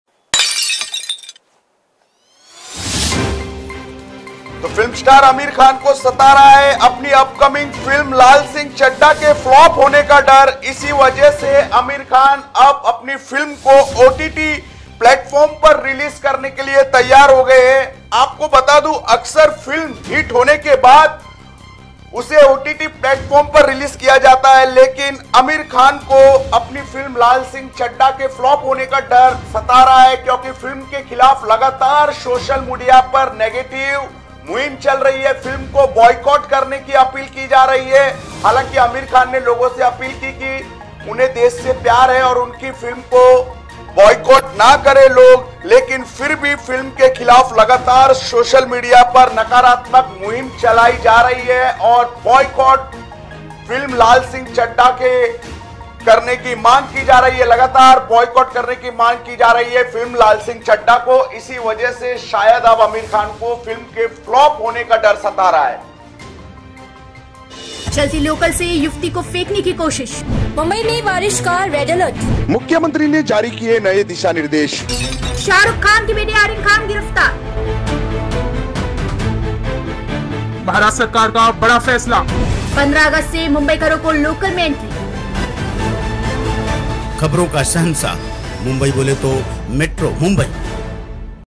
बॉयकॉट की मांग के बीच आमिर खान को सताया फ़िल्म 'लालसिंह चड्डा' के फ्लॉप होने का डर?
बॉयकॉट की मांग के बीच आमिर खान को सताया फ़िल्म 'लालसिंह चड्डा' के फ्लॉप होने का डर? OTT पर रिलीज करने को तैयार